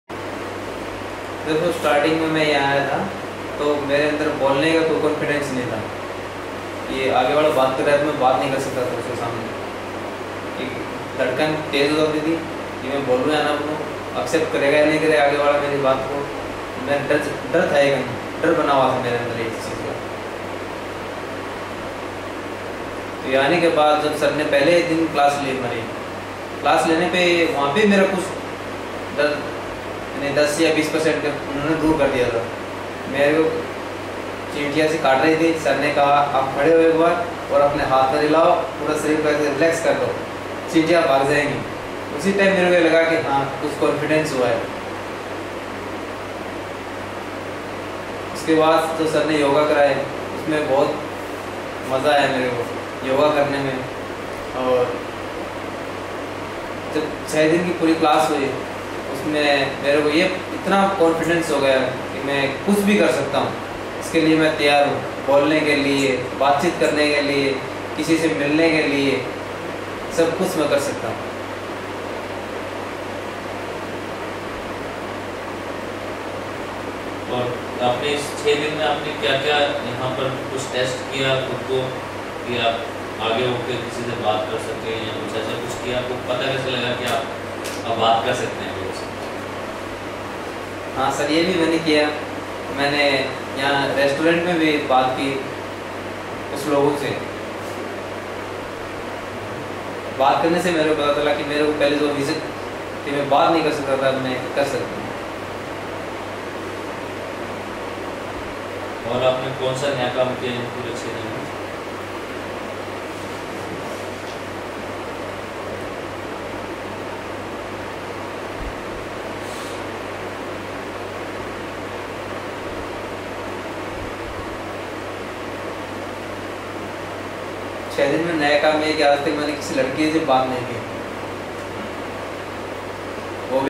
NLP practitioner with ranjit
NLP worked for fear of unknown, height, talking, stage fright, etc